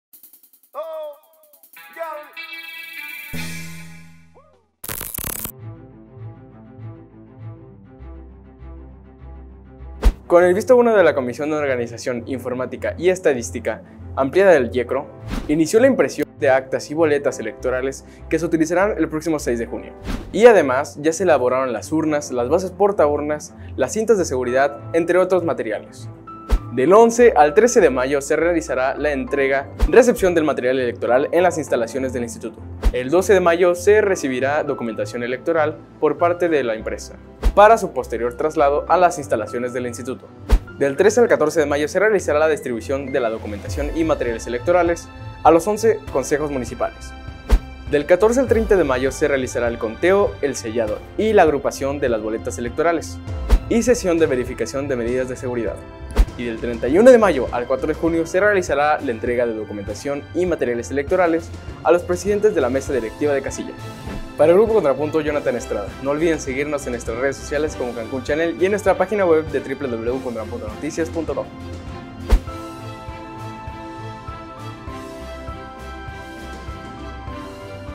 IMPRESIÓN DE DOCUMENTACIÓN Y BOLETAS ELECTORALES
Del 31 de mayo al 4 de junio, se realizará la entrega de la documentación y materiales electorales a las presidentas y presidentes de las Mesas Directivas de Casillas